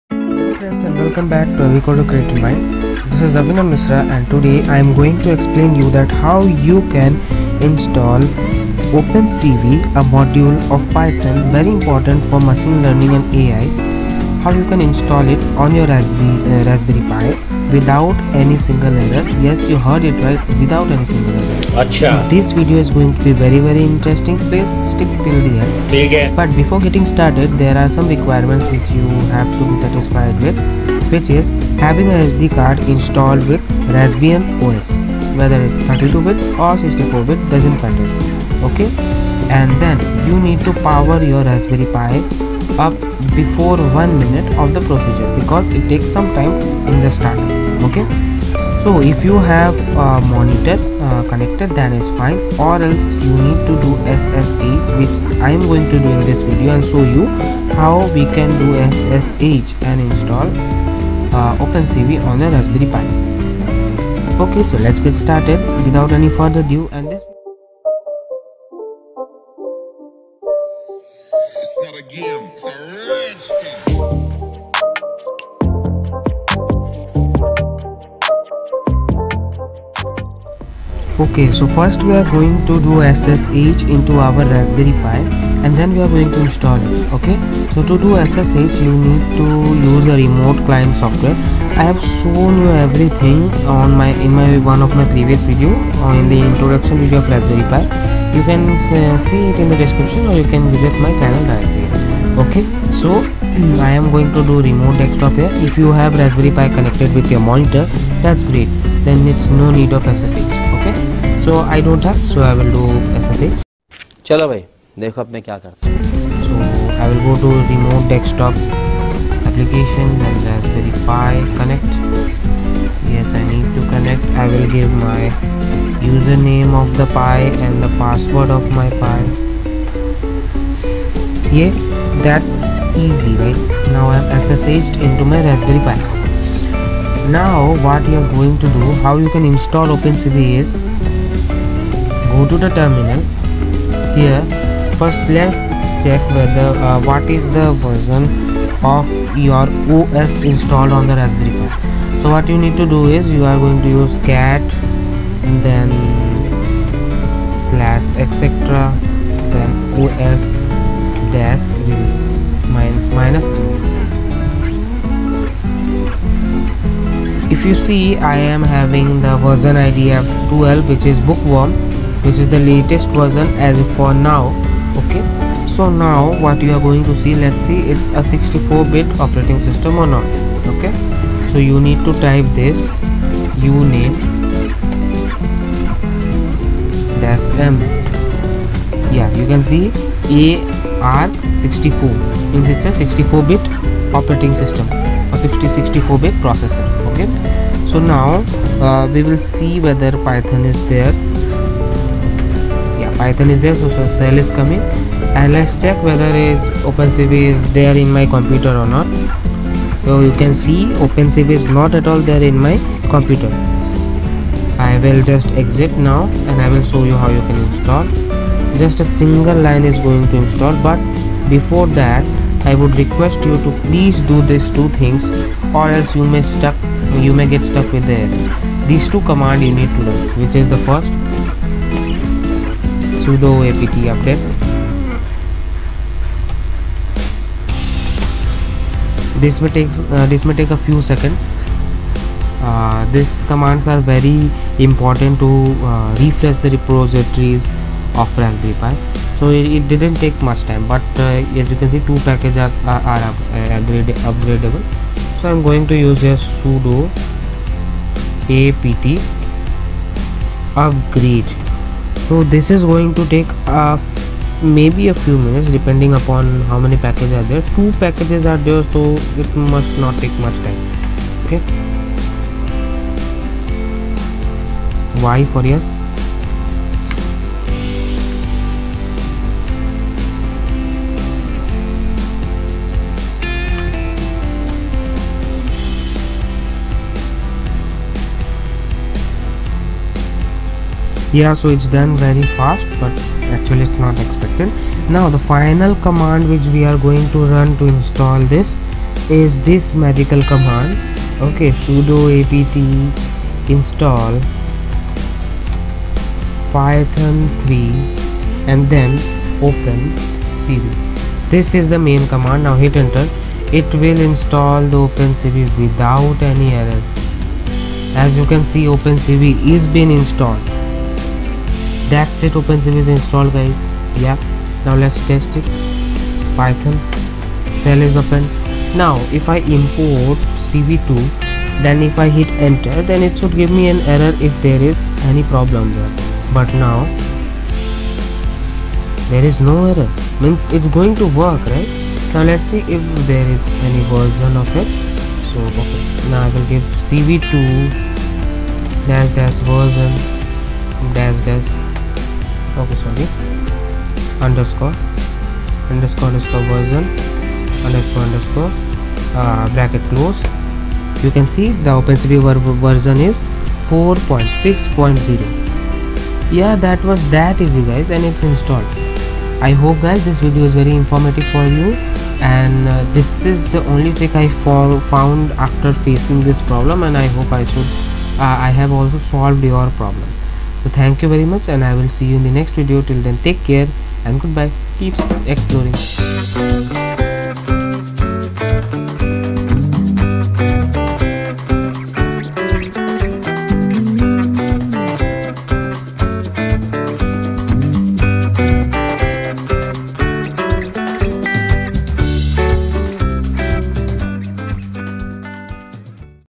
How to install OpenCV on Raspberry Pi without any error | Very easy | Installing OpenCV | Abhicoder
Step 1: Connect your Raspberry Pi to power

Step 2: Connect a monitor or SSH into your Pi

Step 3: Open terminal and run the command, 'cat /etc/os-release'

Step 4: Then this, " uname -m "

Step 5: Then this, " sudo apt update "

Step 6: Then this, "sudo apt upgrade" (May take some minutes!)

Step 7: Run the final magical command, " sudo apt install python3-opencv "
DONE!

Hey guys! Welcome to another video. In this video I have shown you that how you install OpenCV on a Raspberry by without any kind of Virtual Enviroment on the real OS. Keep exploring!